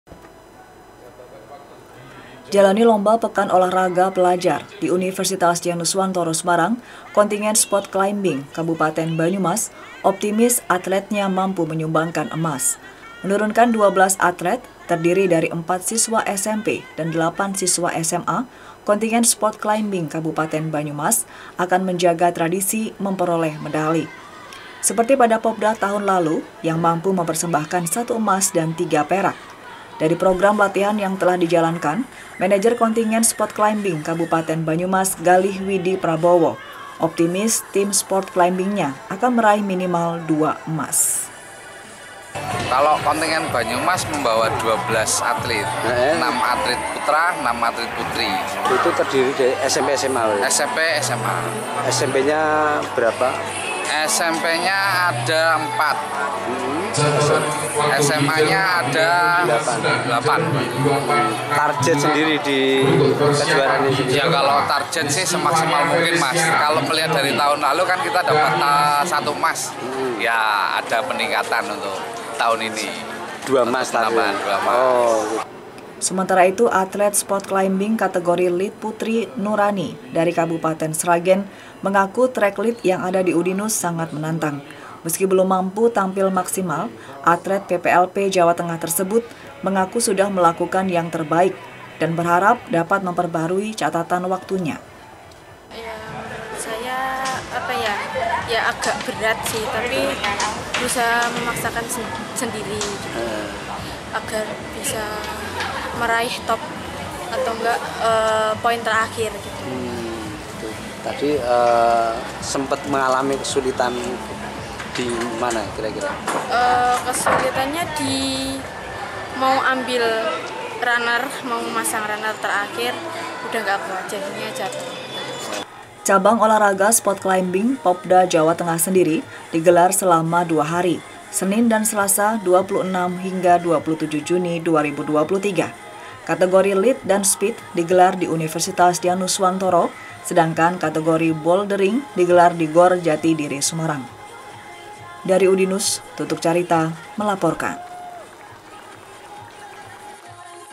Kontingen Sport Climbing Banyumas Optimis Raih 2 Emas
Semarang - Kontingen panjat dinding Kabupaten Banyumas optimis dapat meraih 2 emas pada cabang olah raga sport climbing. Optimisme ini tidak terlepas dari raihan Popda tahun lalu, yang memperoleh satu emas dan 3 perak. (tuk)